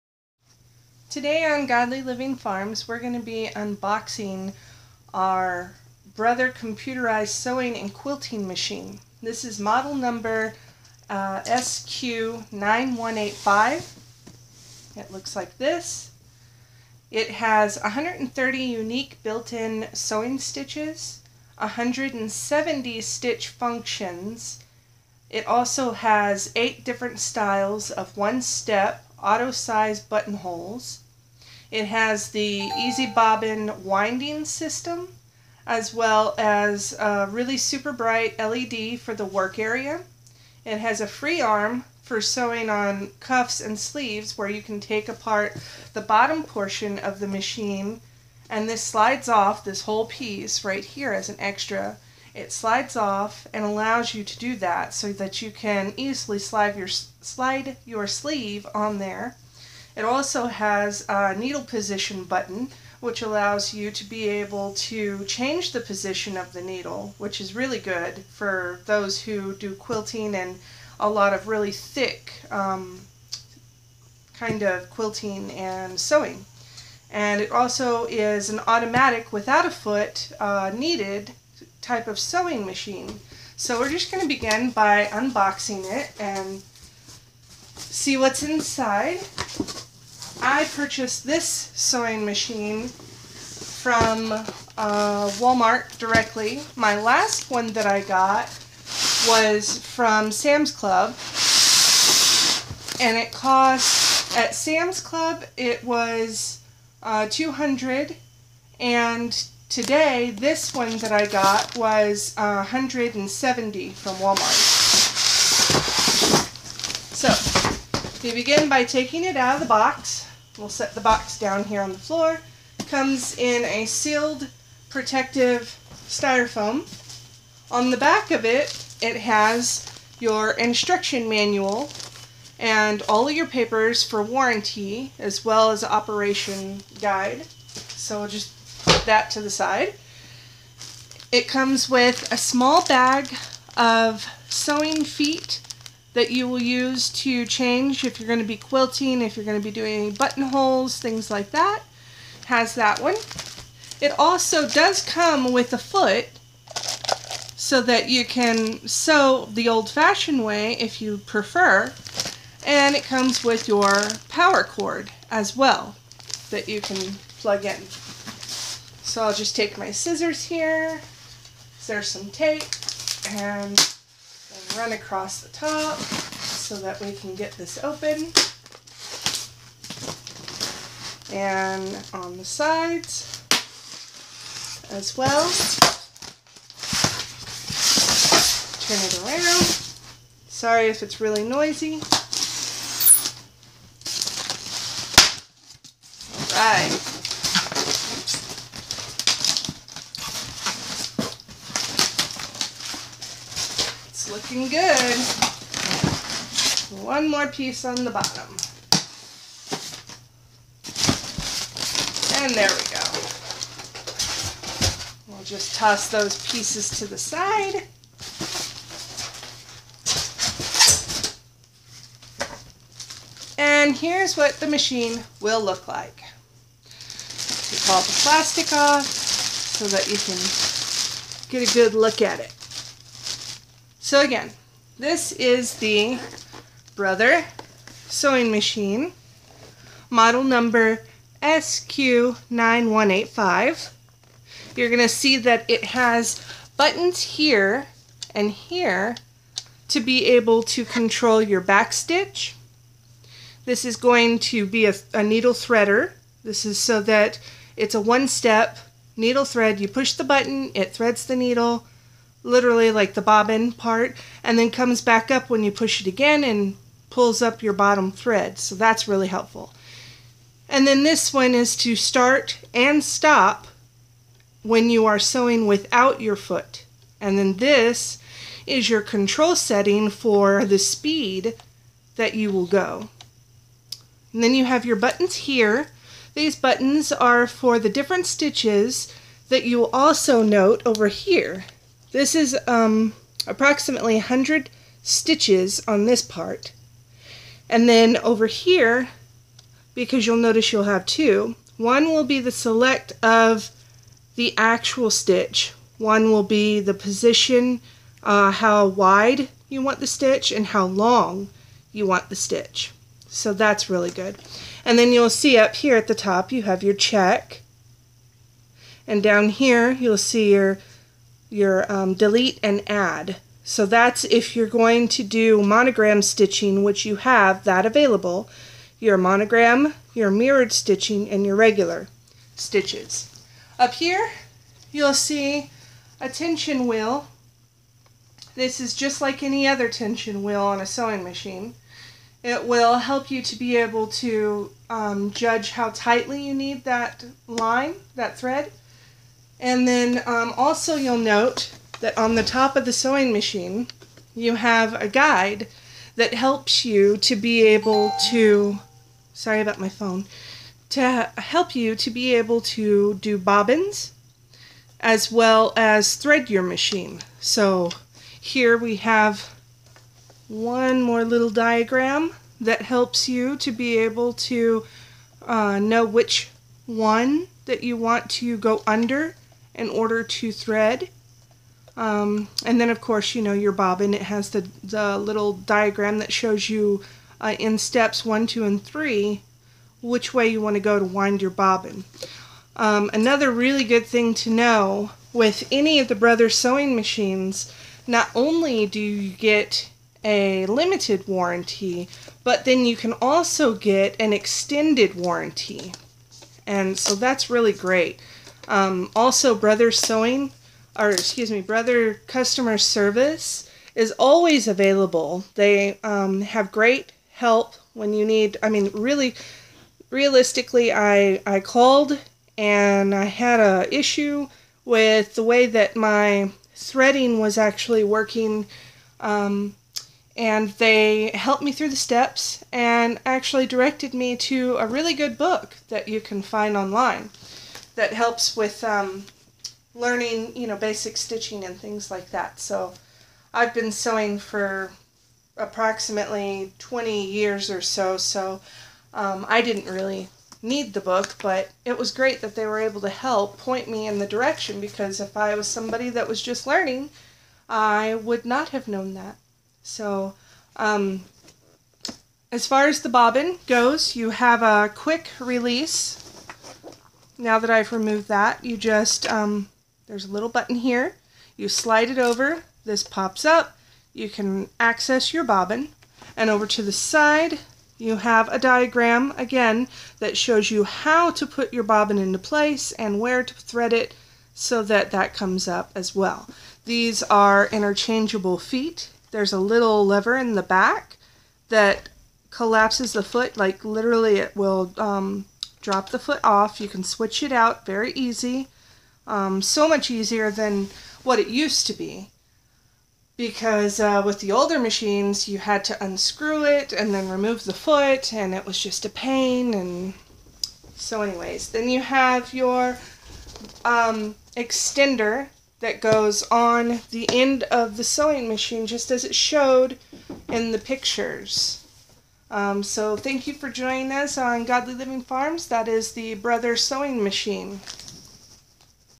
Review for Brother SQ 9185 sewing machine
I describe this machine and what it can do for you. Later I will be posting some demos using this model of brother sewing machine.